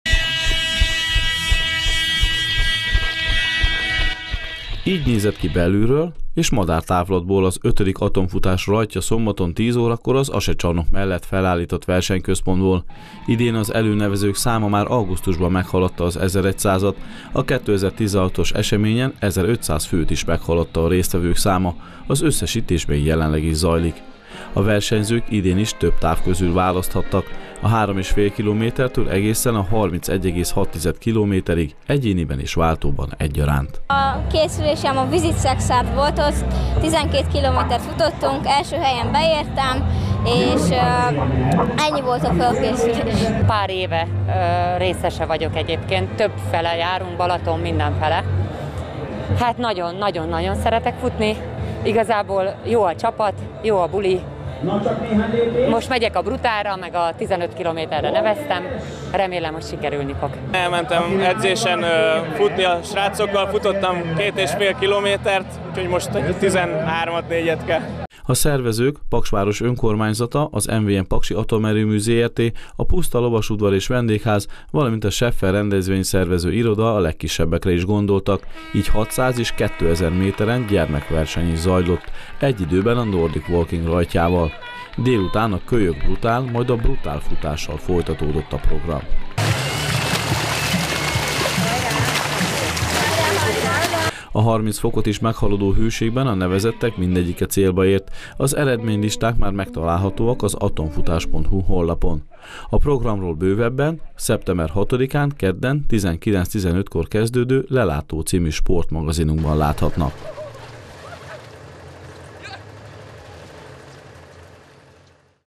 Rekord mennyiségű versenyző volt az idei Atomfutáson
Minden eddiginél több versenyző teljesítette az V. Atomfutás különböző távjait. Az 1500 fő feletti létszám 11 táv közül választhatott, az Atomerőmű sportcsarnok szomszédságában a kisérőprogramok mind magas színvonalon zajlottak, este a Kiscsillag együttes koncertezett.